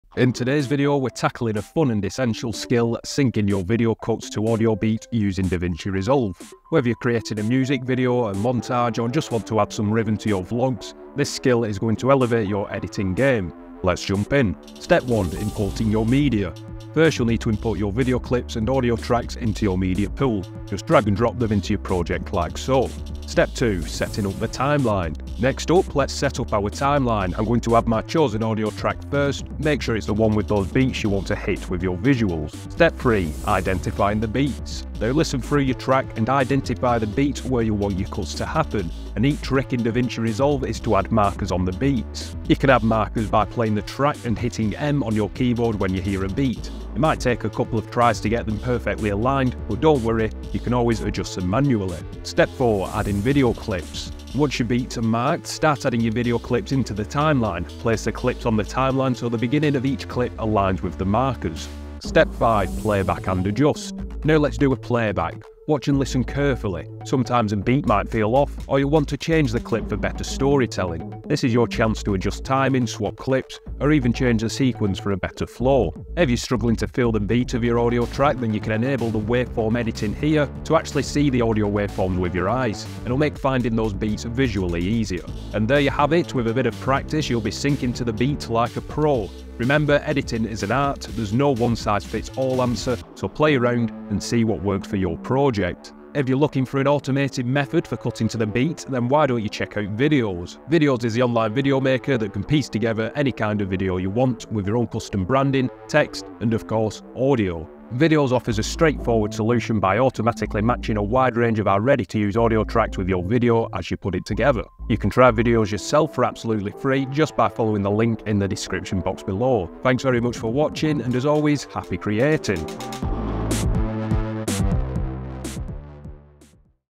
Sync Video Cuts to Music Beats in DaVinci Resolve 2024
Master the art of syncing video cuts to music beats with this engaging tutorial using DaVinci Resolve. Perfect for music videos, dynamic montages, or adding rhythm to your vlogs, this technique is a game-changer in video editing. We'll guide you step-by-step through importing media, setting up your timeline, marking beats, aligning video clips, and fine-tuning for the perfect sync. By the end of this video, you'll have the skills to create videos that not only look good but also feel in tune with your chosen audio.

🕒 Timestamps:

[0:00] Introduction to Syncing Video Cuts
[0:16] Step 1: Importing Media
[0:24] Step 2: Setting Up the Timeline
[0:34] Step 3: Identifying Beats
[0:53] Step 4: Adding Video Clips
[1:03] Step 5: Playback and Adjustments
[1:26] Conclusion and Additional Tips

🧠 What You'll Learn:

Easy steps to align your video cuts with music beats
Tips for identifying and marking beats accurately
Techniques for adjusting and perfecting your sync